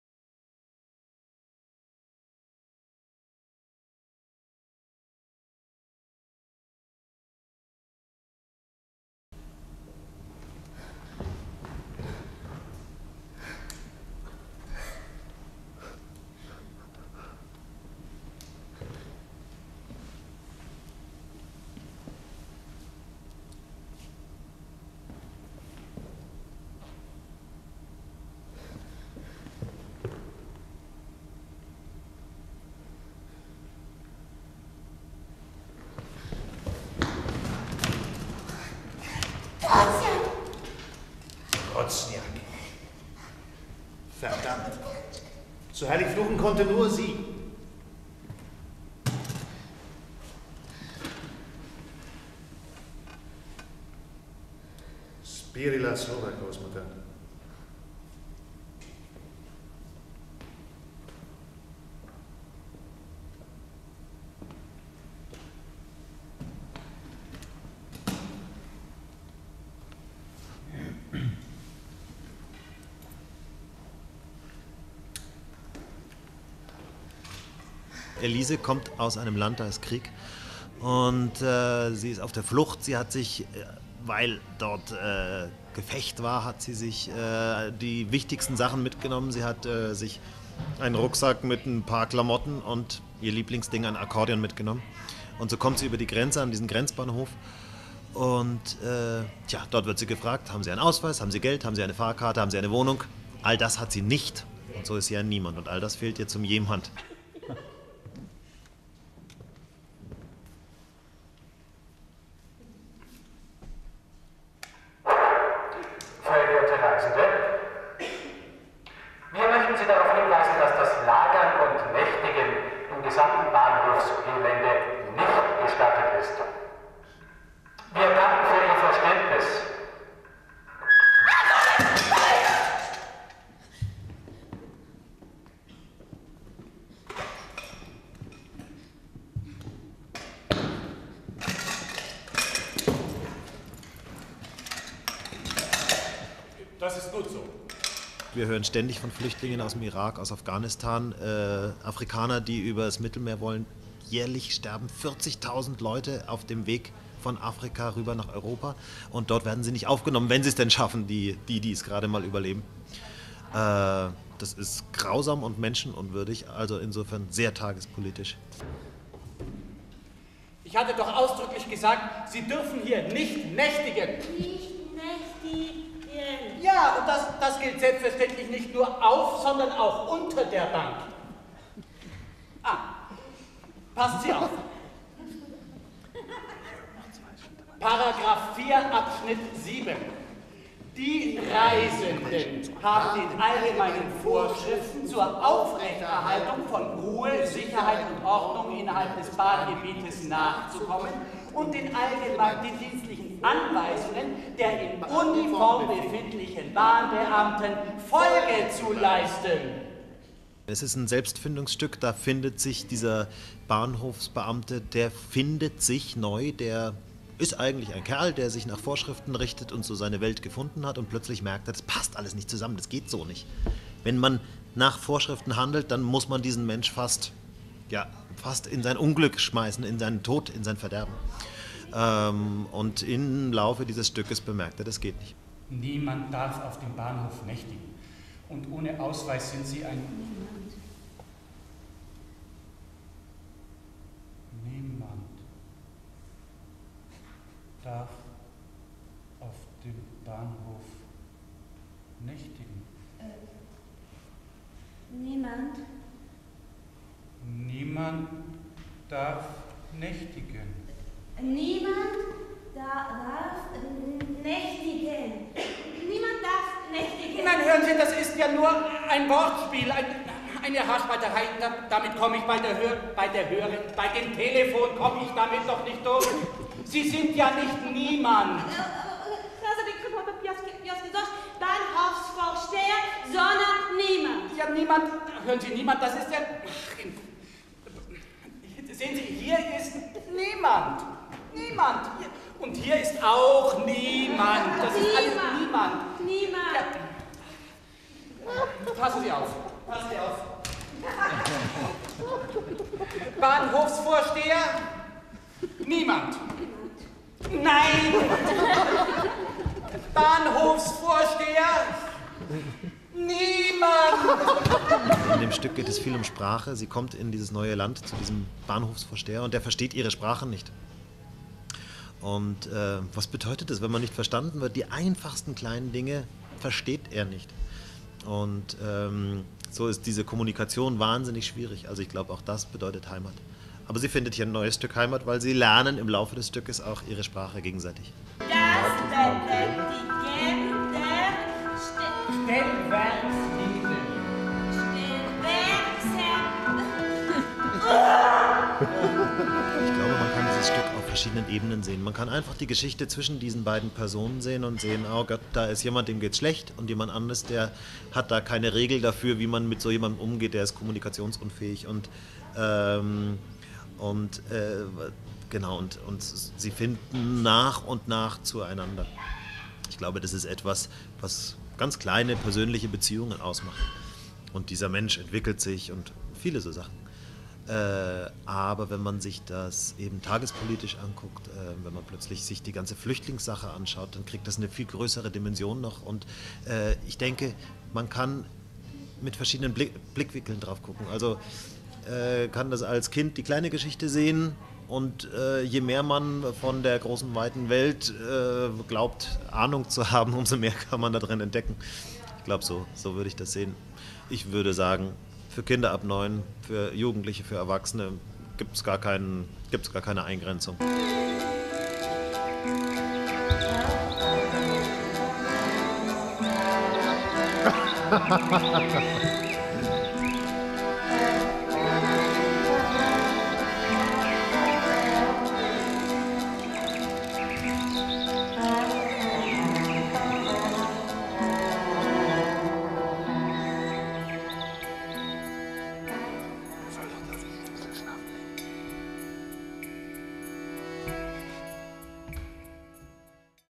Niemand heißt Elise
Für die Internetseiten des Stadttheaters Fürth und des Theaters Pfütze in Nürnberg entstand der Trailer zum Stück "Niemand heißt Elise" im April 2010. Das Schauspiel nach einem gleichnamigen Stück des Theater Metronom für Kinder ab neun Jahren und Erwachsene erzählt über das Wegsollen und das Dableibenwollen, über das Verschiedensein und das Vertrautwerden. Ein Stück über Courage, und über die Liebe.